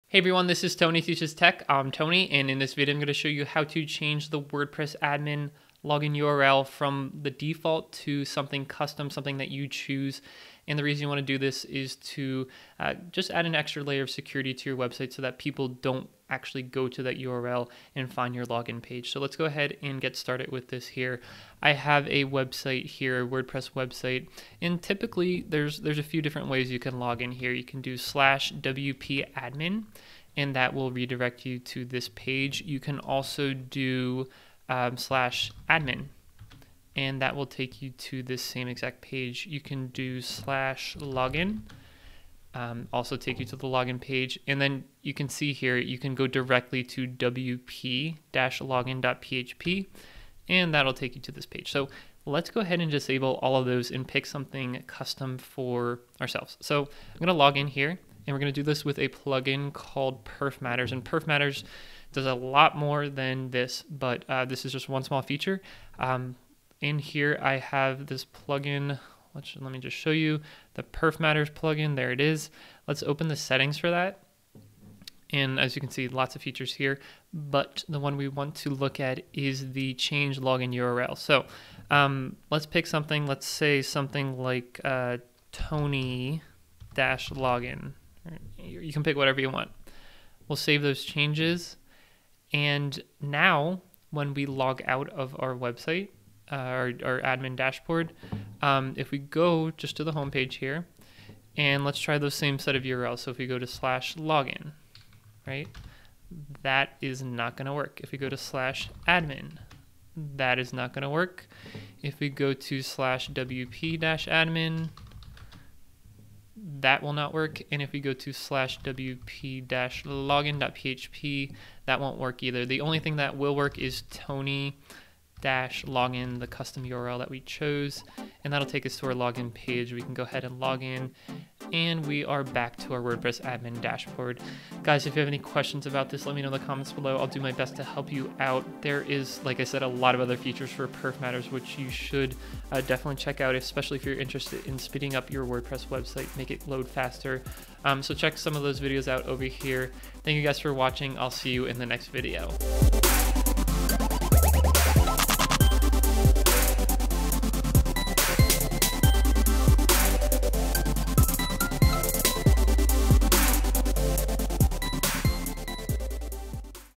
How to Change wp-admin URL in WordPress with Perfmatters
Learn how to change the wp-admin by disabling it and creating a new login URL login.